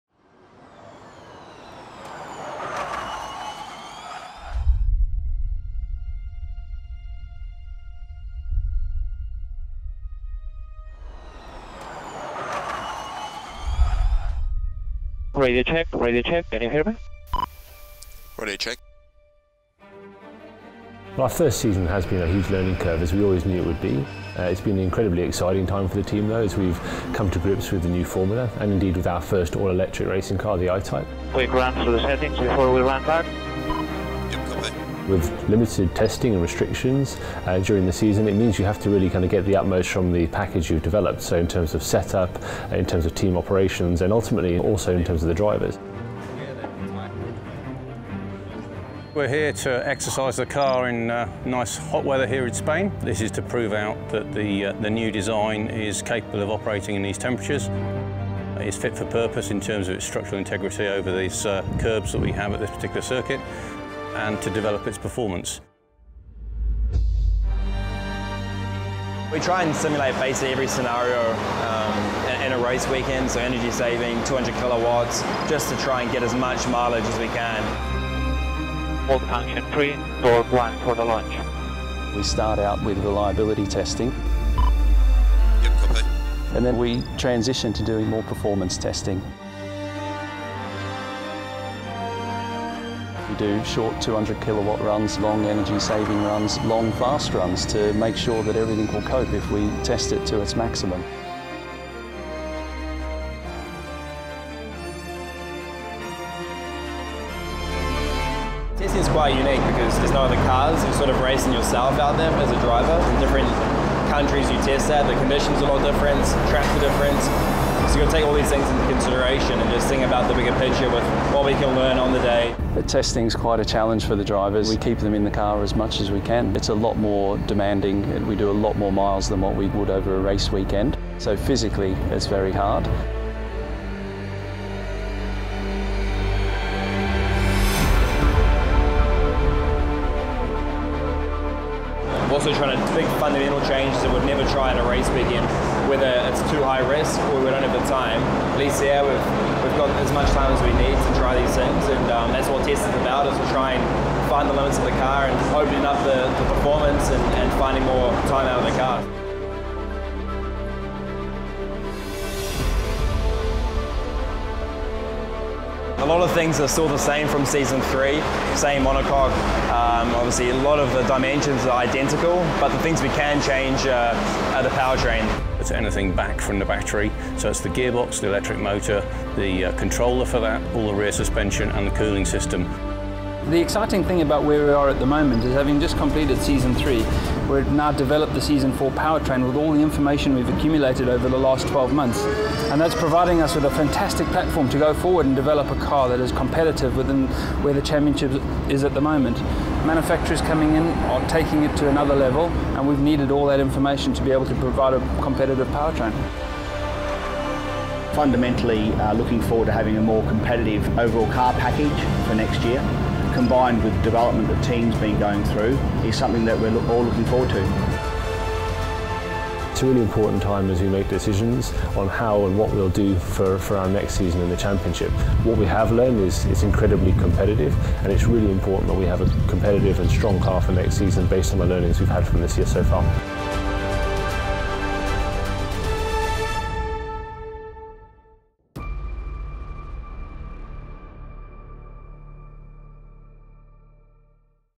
Panasonic Jaguar Racing | I-TYPE Testing in Spain
Panasonic Jaguar Racing return to the track ahead of season four of the FIA Formula E Championship, as they put the I-TYPE 2 through its paces in Spain.